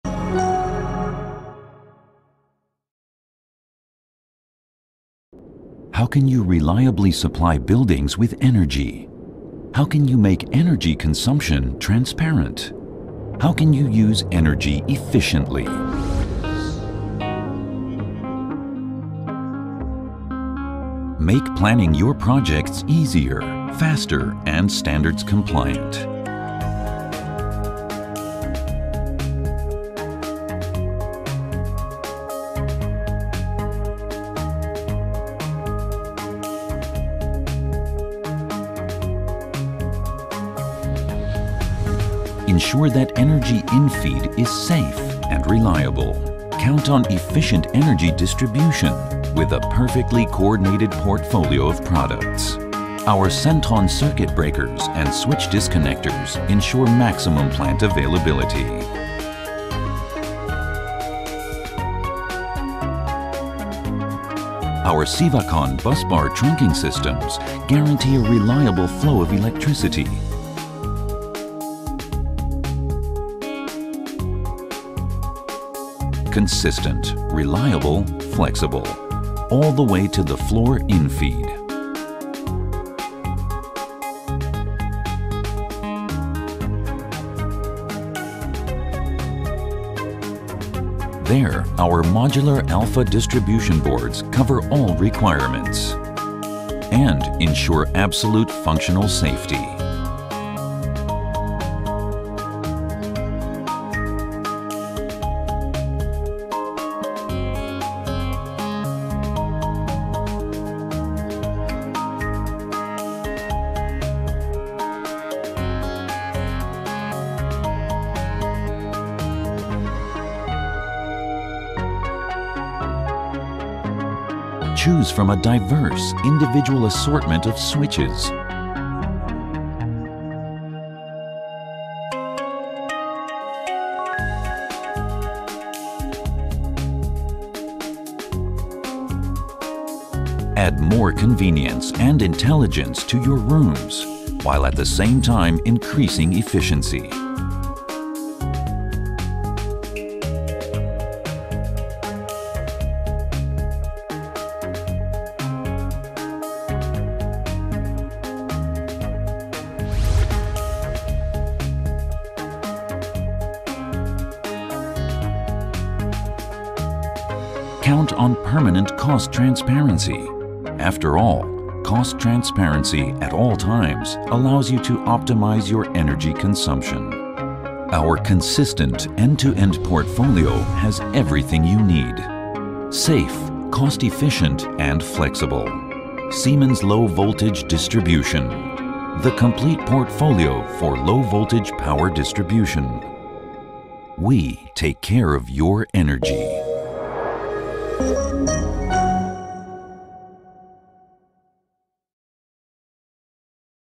سیستم توزیع فشار ضعیف
معمولا در ورودی اشتراک برق و پس از کنتور از تابلو های توزیع اصلی برای پخش کردن انرژی در ساختمان یا کارخانه استفاده می شود. در تابلو_توزیع برق در سیستم های فشار_ضعیف از تجهیزاتی مانند بریکر؛ فیوز؛ کلید_فیوز؛ باسداکت و باسبارها استفاده می شود. هم خوانی این تجهیزات و قابلیت اتصال آن ها به یکدیگر باعث افزایش کیفیت و قابلیت اطمینان می شود.